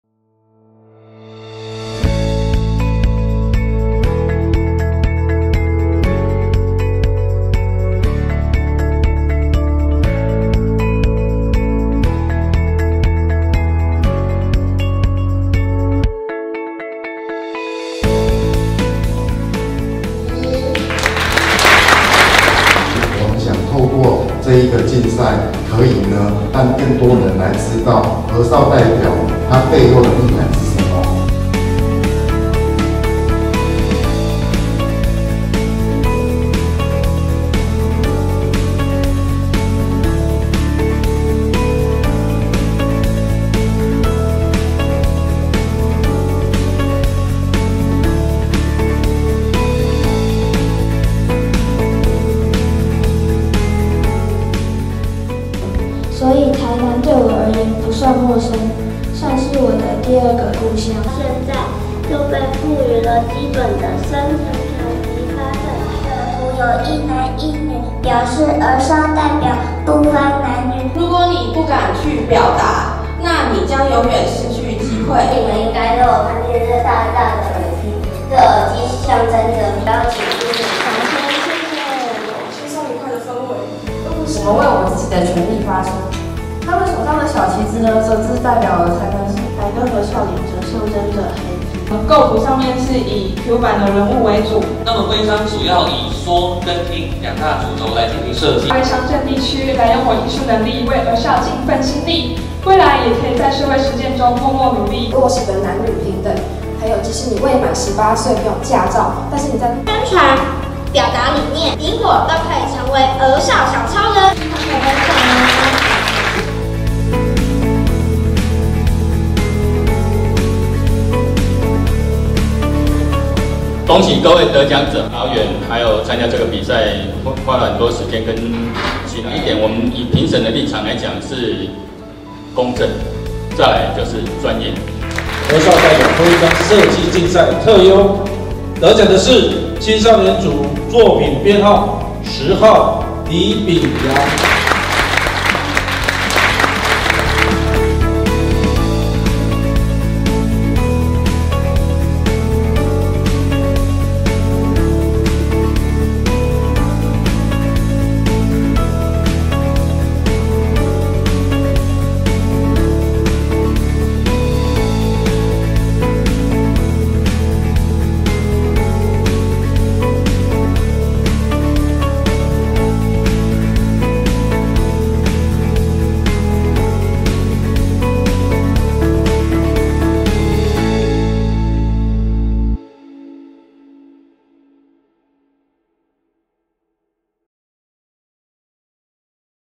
【我是兒少 代表發聲】「臺南市兒少代表徽章設計徵件競賽」總決賽精彩花絮
每一個作品都有獨一無二的創作理念與故事📔
最適合說故事的人就是 創作者 🖌
「『我是兒少 代表發聲』臺南市兒少代表徽章設計徵件競賽」總決賽，我們邀請入圍的參賽者們一起齊聚一堂，將每個作品的獨特故事分享給大家🗣，同時評選出特優、優選、佳作🏆，無論獲得什麼名次，每個作品都是獨特且最棒的🥰
就讓我們用影像回到當天的精彩盛況吧！